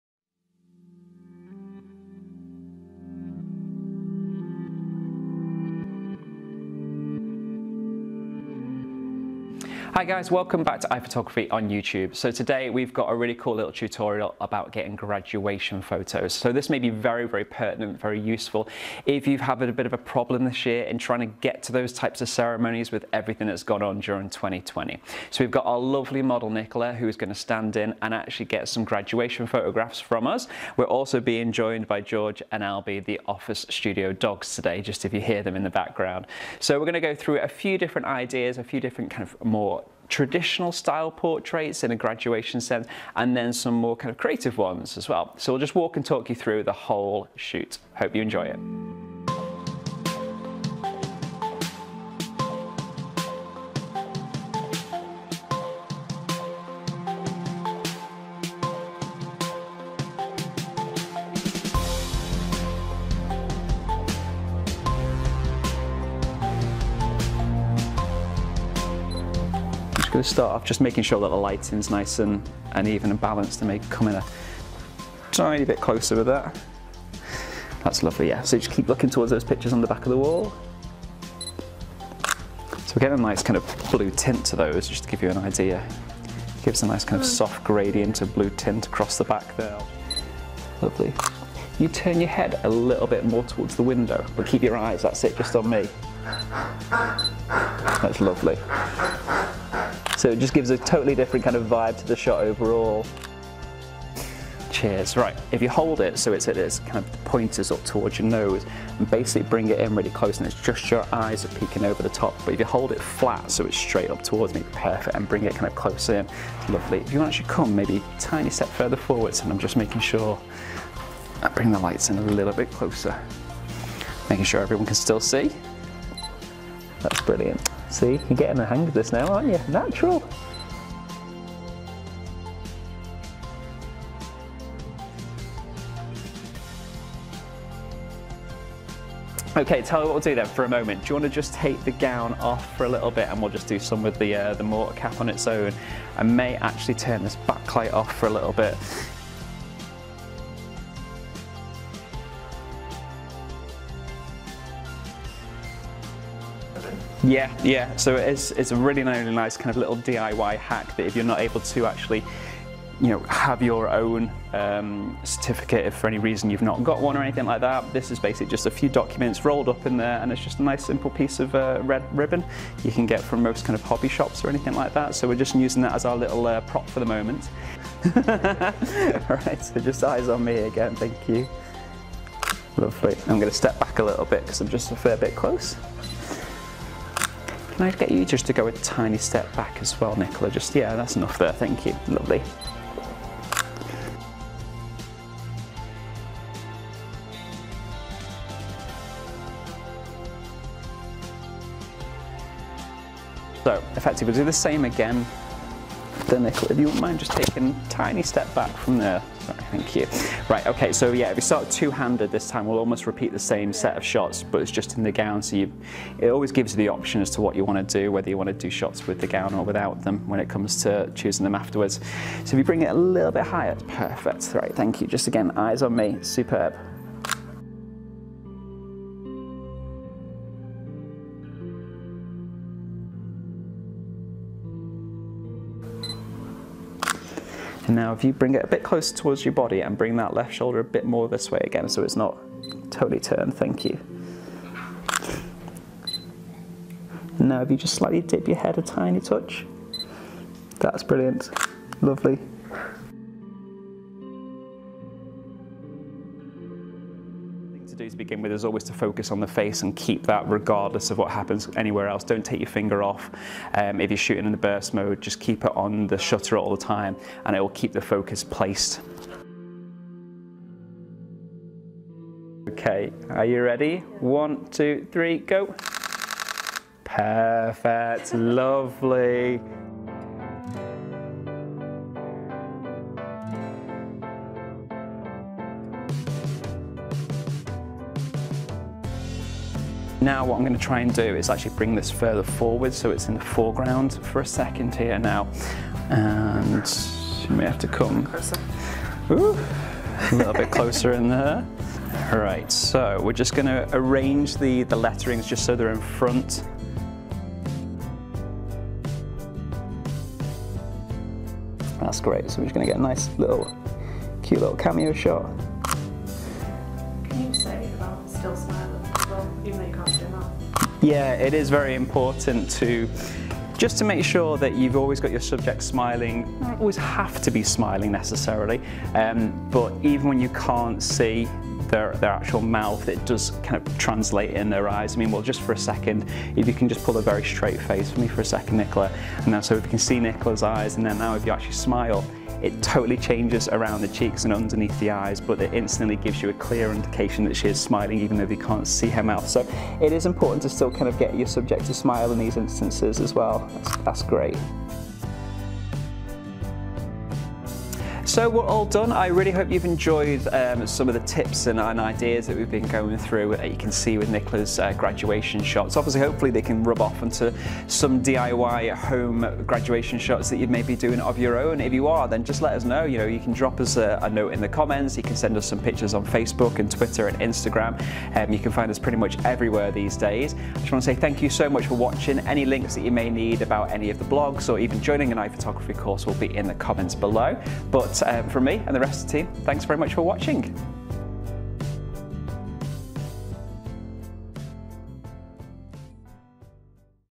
Graduation Photography for Beginners PART 1
COVID-19 has cancelled pretty much everything. Nicola got in touch with iPhotography and asked us to take some Graduation shots in place of the ceremony she never had.